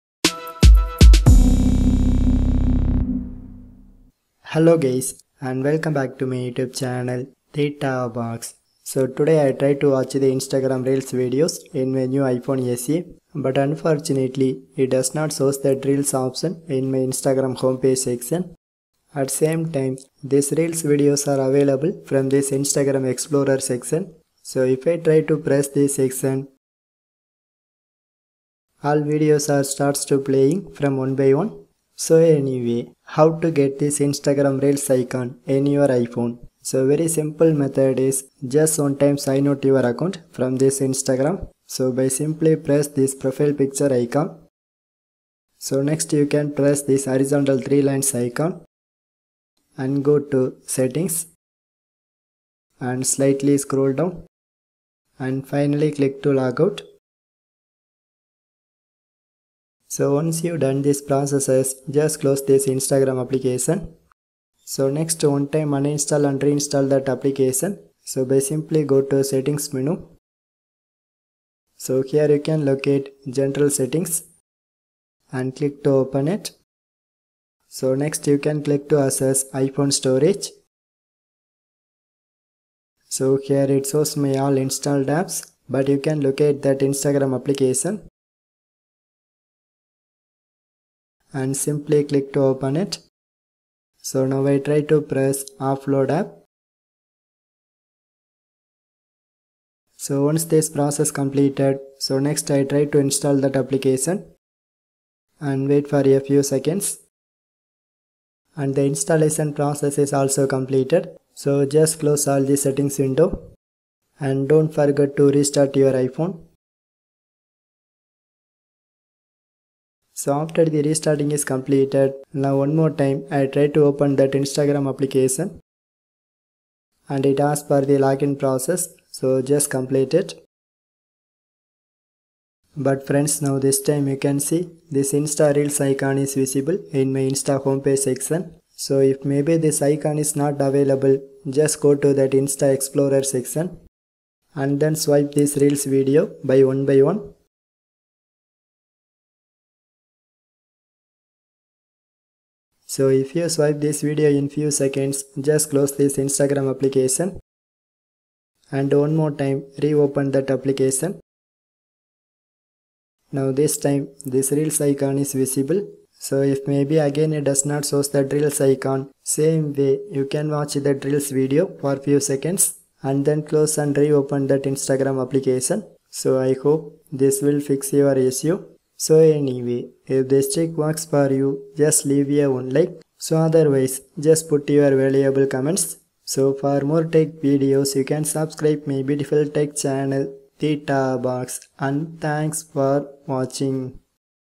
Fix instagram reels not showing on iPhone | Add reel back to profile grid | Reels missing
This tutorial is about how to fix instagram reels option not showing up or missing | get reels back to profile grid.

This solution only works on ios 15.2 based iPhone devices.

This video posted on december 2022,but in future 2023 if may be they release any new update this will work for you.

But some time this method not working for ipad devices.

Some working countries are india,nepal,pakistan,bangladesh and canada.

This video has english subtitle at same time you can translate to tamil,kaise kare hindi,bangla telugu,sinhala,bengali,kannada and malayalam languages.

how to add reel back to profile grid after removing iphone :

1.One of my subscriber ask why it doesn't show the insta reels option in my feed section.

2.This issue also occurs even after update the new version of instagram app.

3.So very simple method to solve this problem is just one time logout your account.

4.And also delete this entire app or select "Offload App".

5.Once you done this processes,just one time reinstall that application.

6.Now one time you can try to open that insta app and login your profile.

7.If may be this reel feature not available in home page section,just watch reels story videos by one by one.

8.So i hope now this time this problem is solved in permanently and you get the reels option.